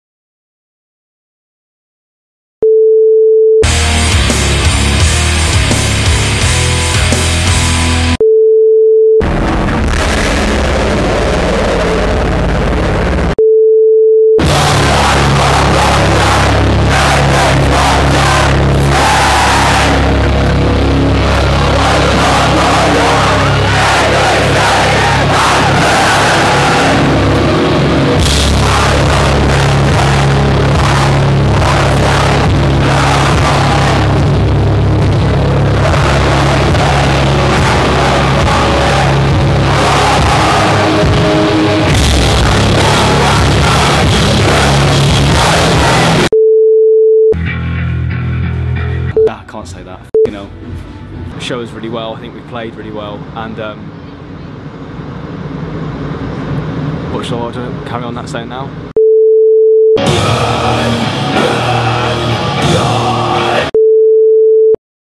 Five Reasons To See Heights On Tour With Biohazard
UK metallic hardcore upstarts Heights are supporting HxC legends Biohazard on their UK tour - here's why you should get there early!